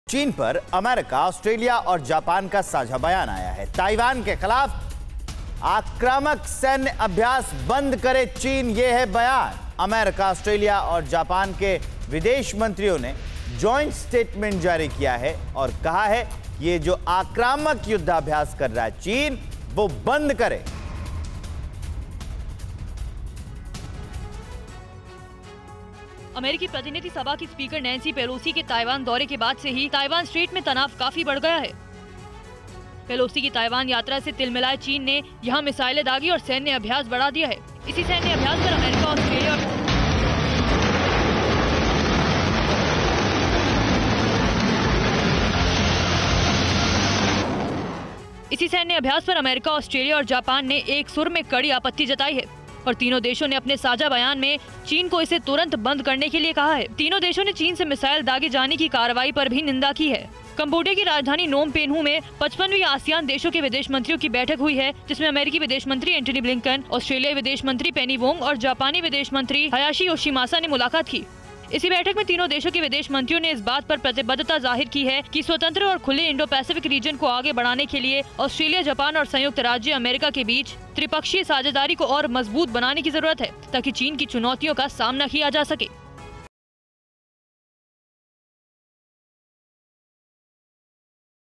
China को सुधर जाने की चेतावनी, वरना 3 देश पड़ेंगे भारी | Taiwan-China Conflict
अमेरिकी प्रतिनिधि सभा की स्पीकर नैंसी पेलोसी की ताइवान दौरे के बाद से ही ताइवान स्ट्रेट में तनाव काफी बढ़ गया है. पेलोसी की ताइवान यात्रा से तिलमिलाए चीन ने यहां मिसाइलें दागी और सैन्य अभ्यास बढ़ा दिया है। इसी सैन्य अभ्यास पर अमेरिका, ऑस्ट्रेलिया और जापान ने एक सुर में कड़ी आपत्ति जताई है और तीनो देशों ने अपने साझा बयान में चीन को इसे तुरंत बंद करने के लिए कहा है. तीनों देशों ने चीन से मिसाइल दागी जाने की कार्रवाई की भी निंदा की है ।